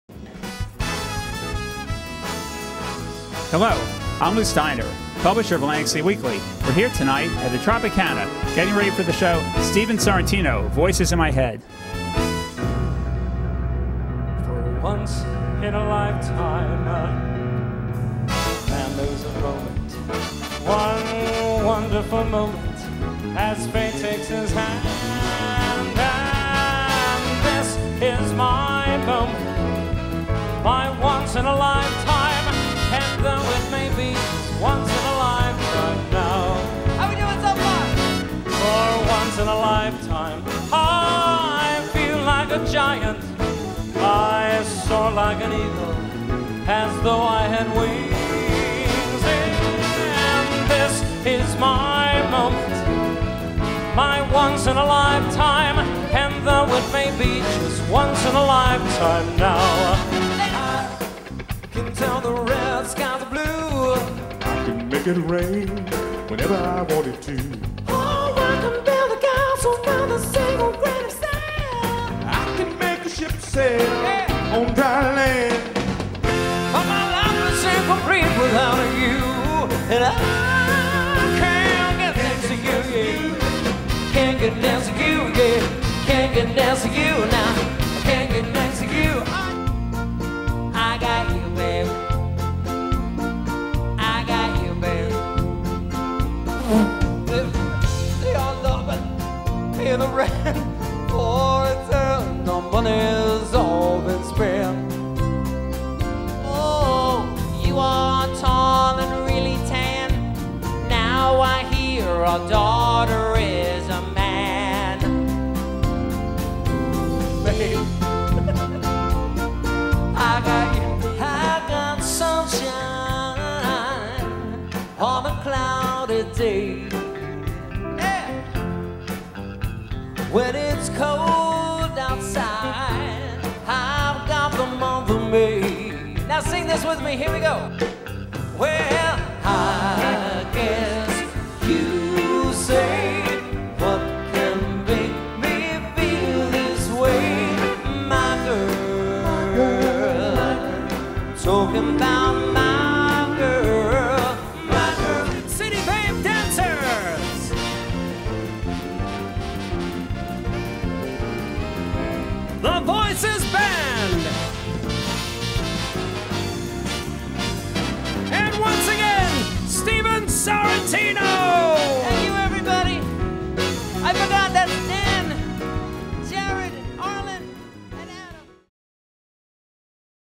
Tropicana Hosts Stephen Sorrentino in AC.m4v
Live on stage at the Tropicana, Atlantic City, is the 3 time Award Winning Musical Comedian, Stephen Sorrentino. Bringing his outlandish variety show to life with "Voices In My Head". This variety show, showcases Stephen's high octane energy along with his sassy comedic style. This fast pace show features lavish showgirl dancers, witty humor and musical impersonations of renowned celebrates. Video edited by Christine Steiner. Video Produced by Lew Steiner, Publisher of Atlantic City Weekly and ACWeekly.com.